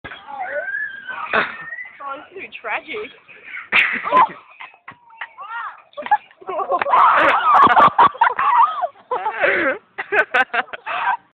James Andrews Extreme Swinging
Swining isn't all its cracked up to be, its an extremely dangerous sport which takes years of practice